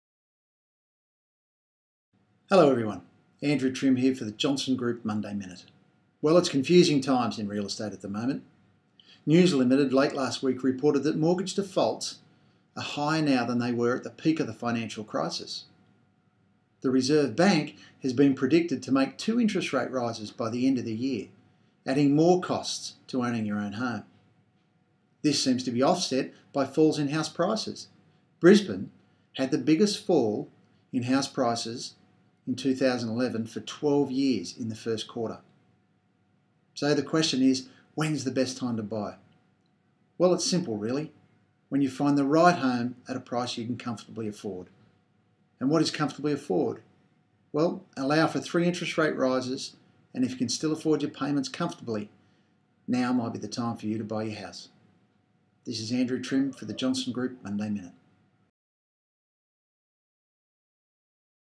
Johnson Real Estate Monday Minute 9th May 2011
A one minute information video on current residential real estate issues. This week - it is confusing times in real estate at the moment. With interest rates set to rise before the end of the year and house prices falling. So when is the best time to buy? When you find the right home and you can comfortably afford it.